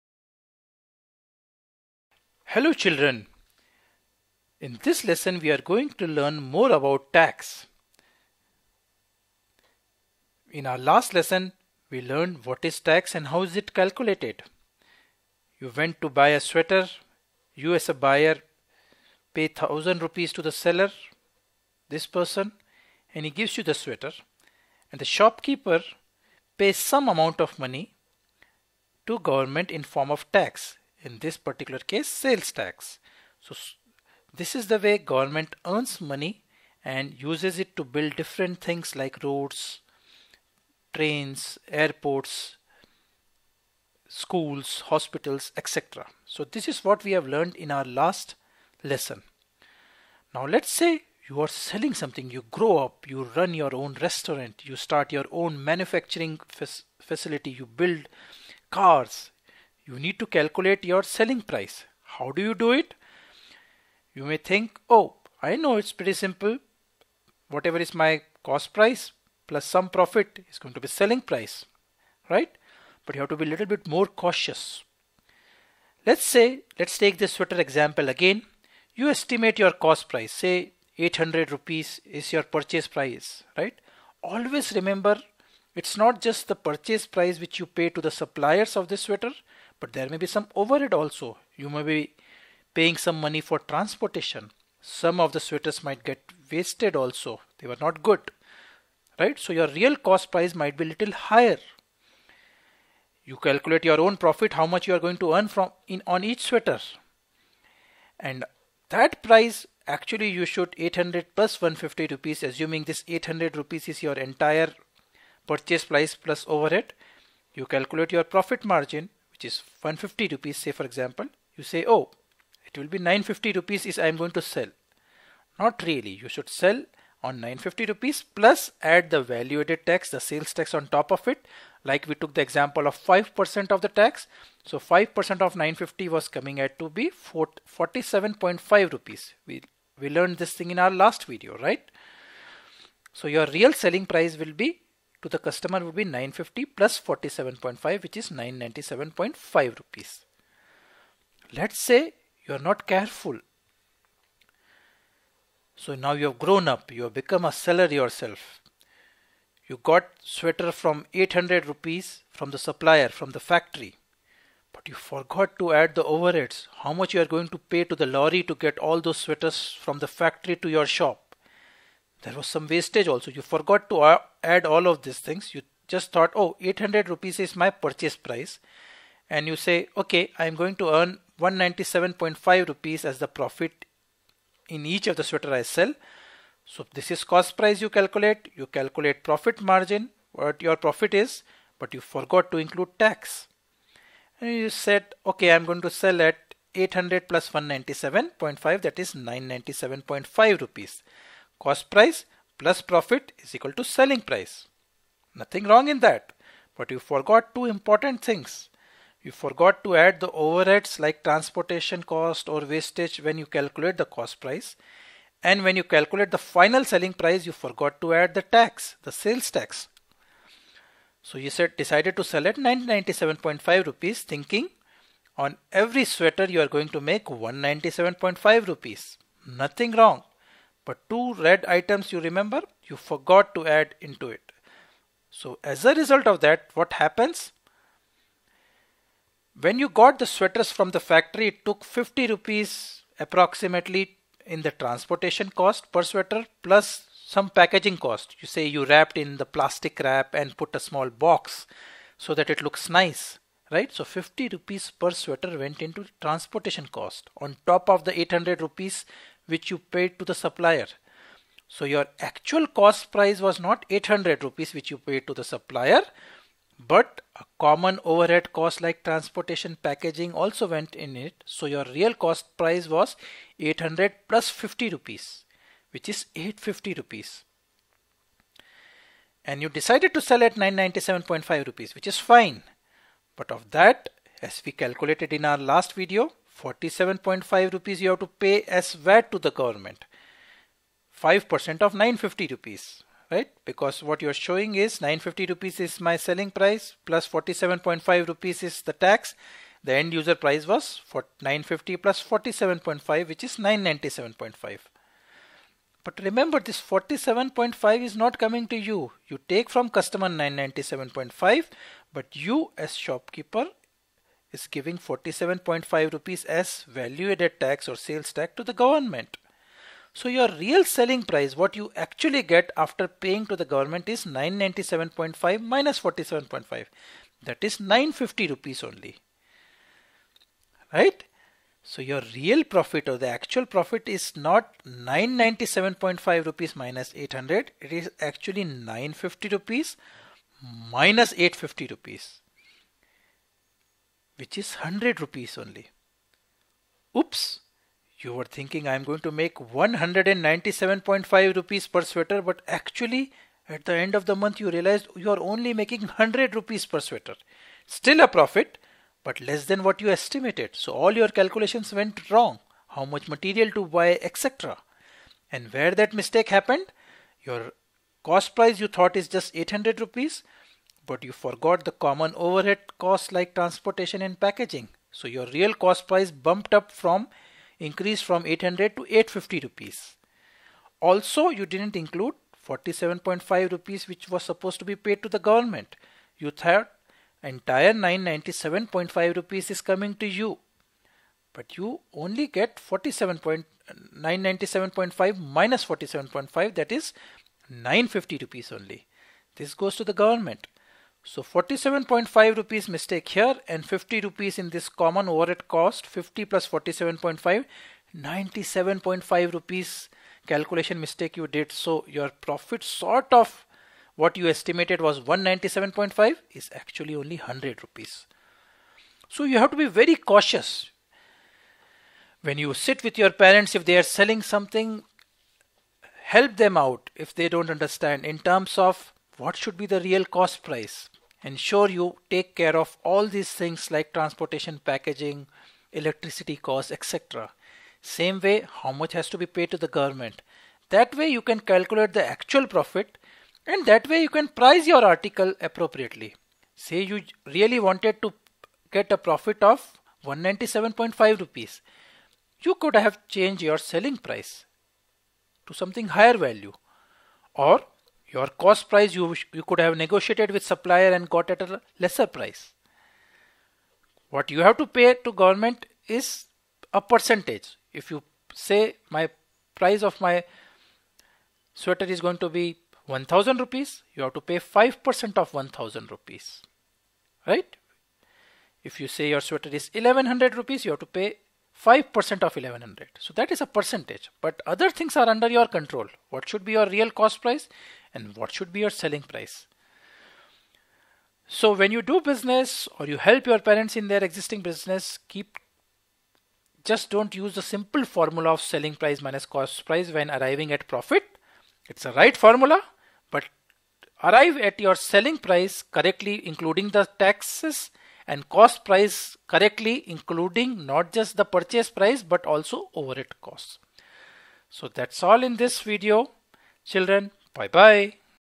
Maths - Understanding tax and profit-loss in business - English
Hello,
BodhaGuru Learning proudly presents an animated video in English which explains how to calculate taxes. It also shows how to calculate profit-loss when tax is included in the frame. It explains finding payable tax, profit and loss percentage etc.

Team BodhaGuru